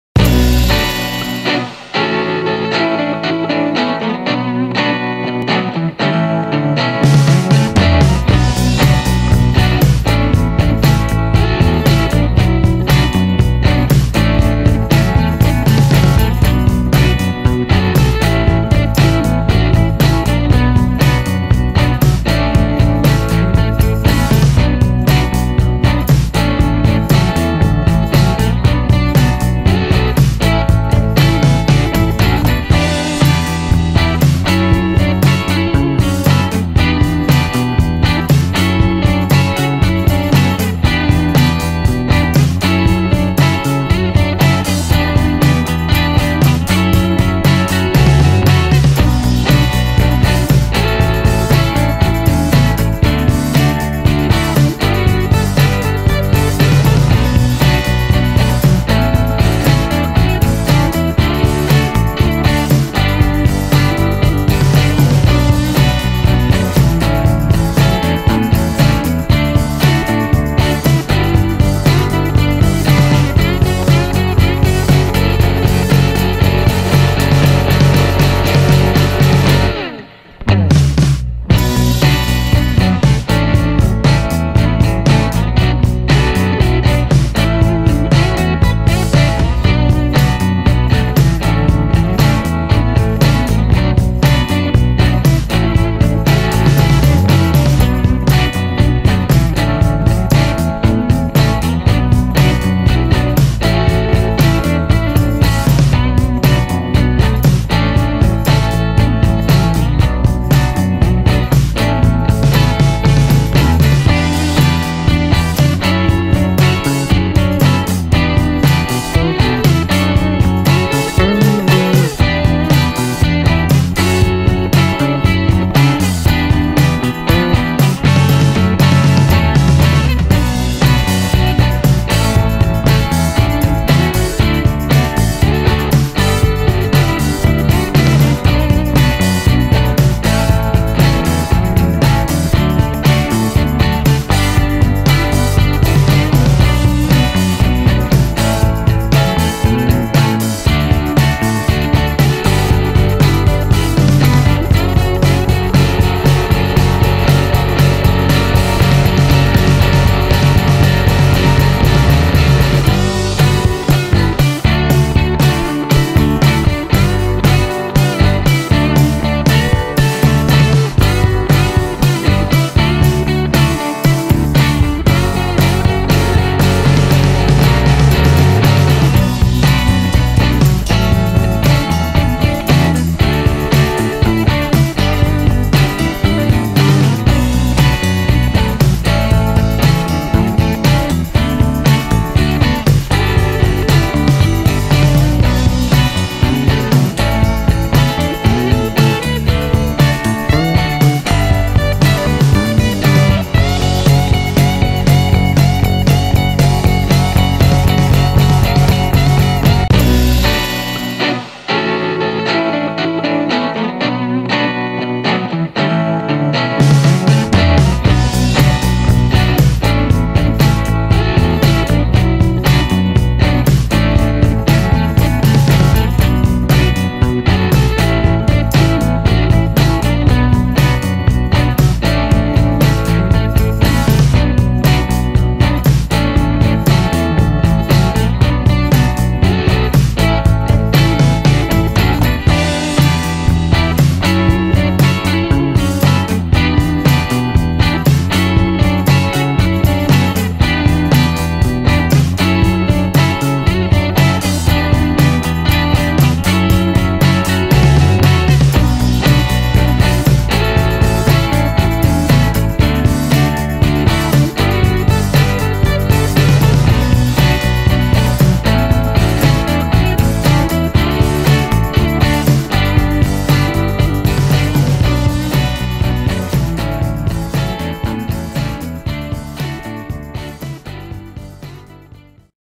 Kia Quoris Kia K900 Running Footage and Interior Views
Автомобиль Kia Quoris, Kia K900 модель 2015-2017